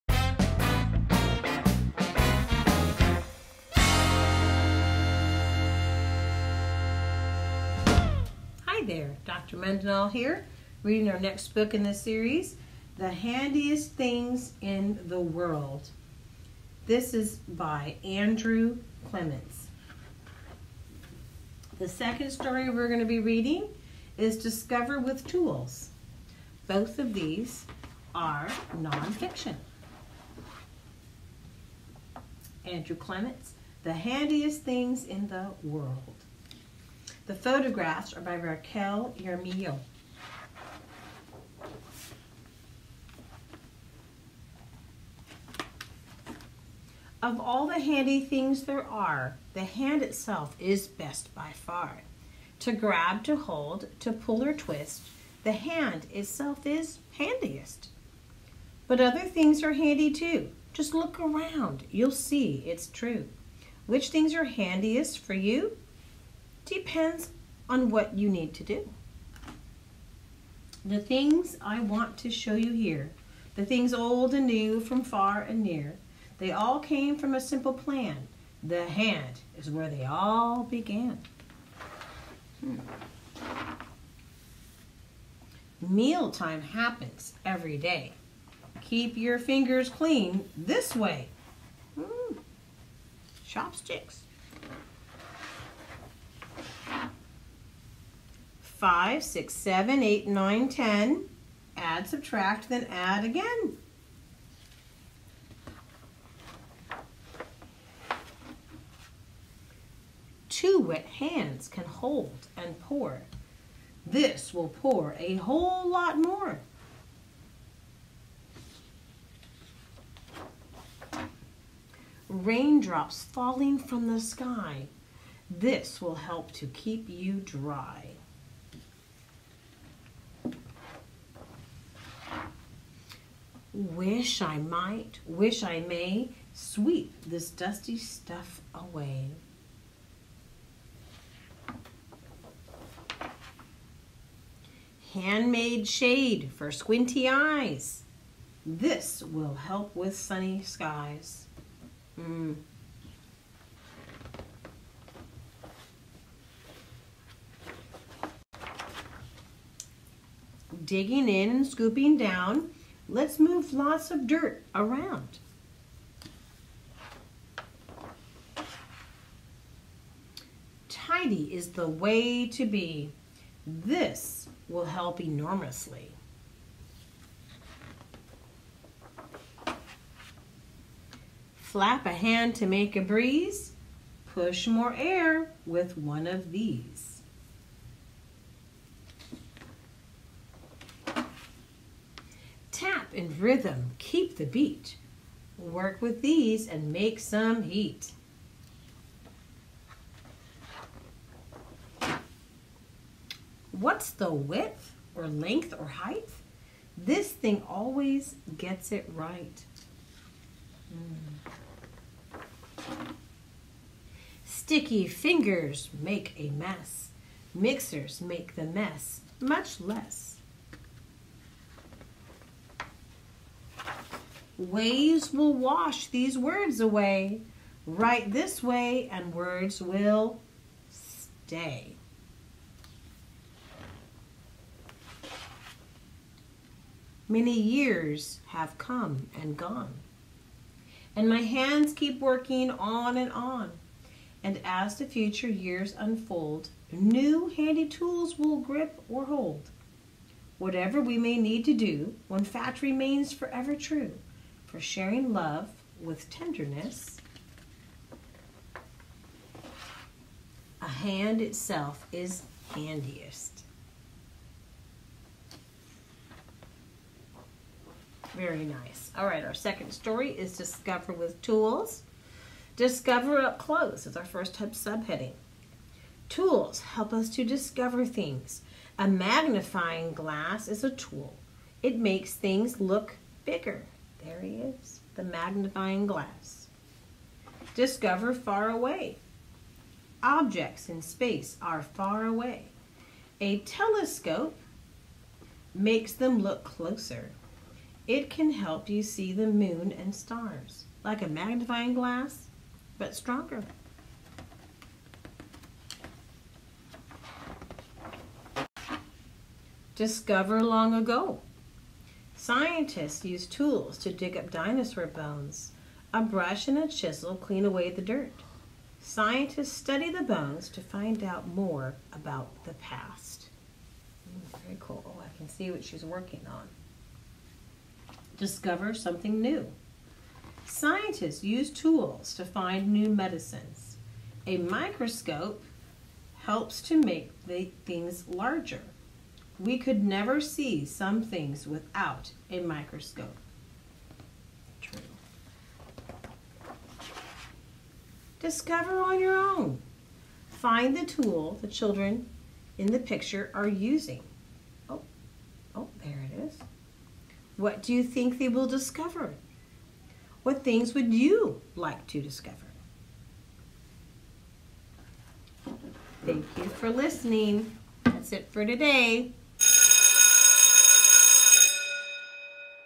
Story 4, 'The Handiest Things in the World'
A famous story written by Andrew Clements, a poem really, that is being read-aloud to give students access to the curriculum of Wonders.